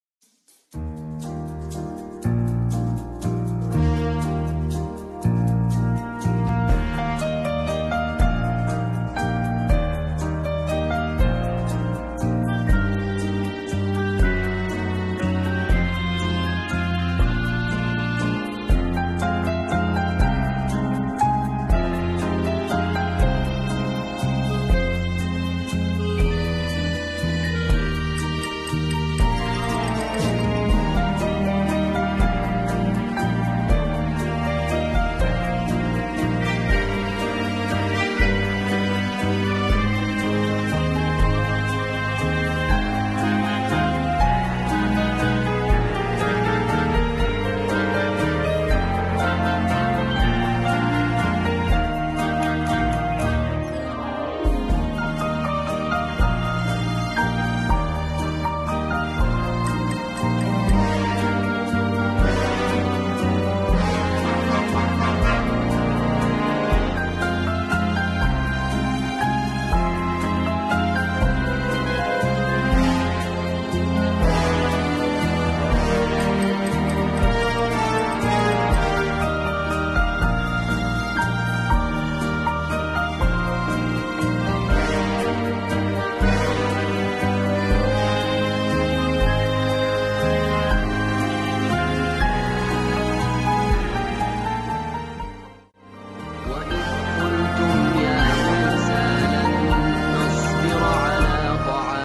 Lomba peragaan busana dan Murotal di SMPN 4 Rangkasbitung
Cuplikan lomba peragaan busana dan Murotal di SMPN 4 Rangkasbitung